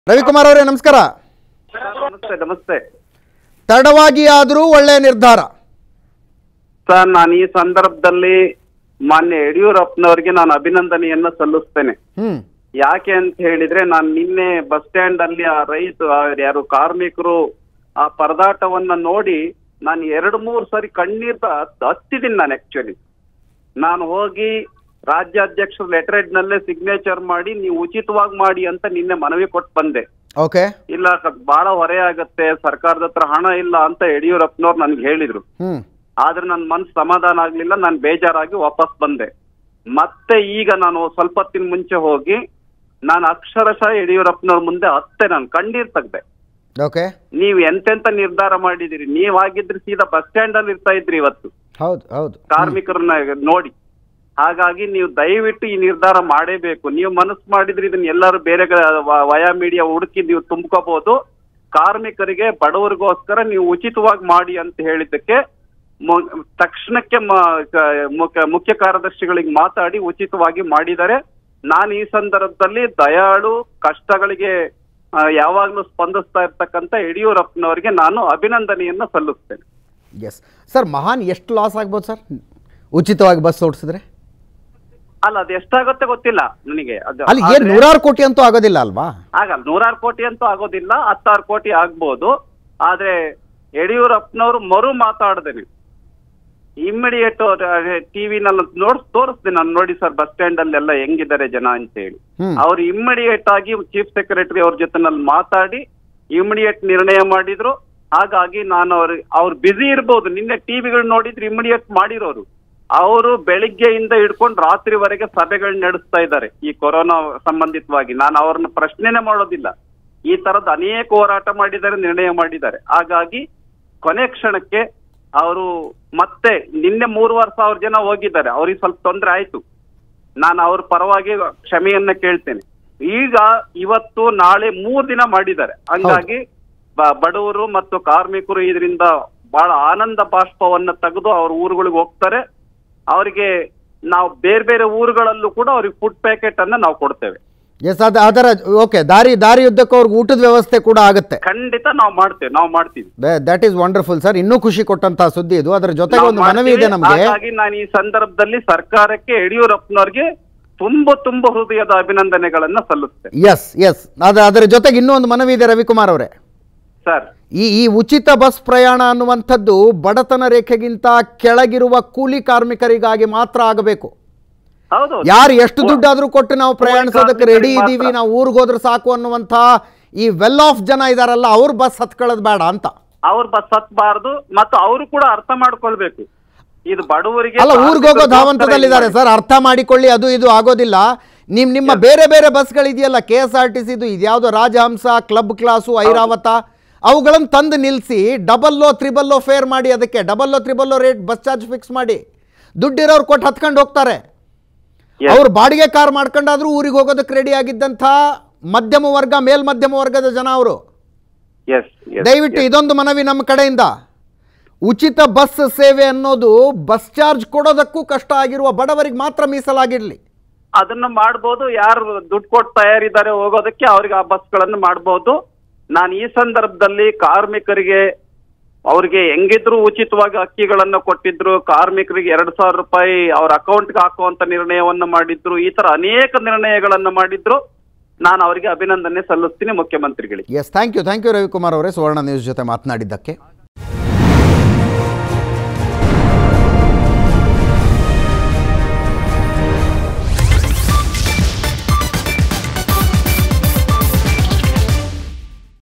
ಬಸ್‌ಸ್ಟ್ಯಾಂಡ್‌ನಲ್ಲಿ ಕಾರ್ಮಿಕರ ಸ್ಥಿತಿನೋಡಿ ಕಣ್ಣೀರು ಬಂತು, ಸಿಎಂ ಮುಂದೆ ಅತ್ತು ಬಿಟ್ಟೆ: ಬಿಜೆಪಿ ಶಾಸಕ ರವಿಕುಮಾರ್
After facing flak for charging exorbitant fares to transport stranded migrants, the State Government on Sunday morning announced that they will ferry migrants to their respective districts without any fare for three days starting from Sunday.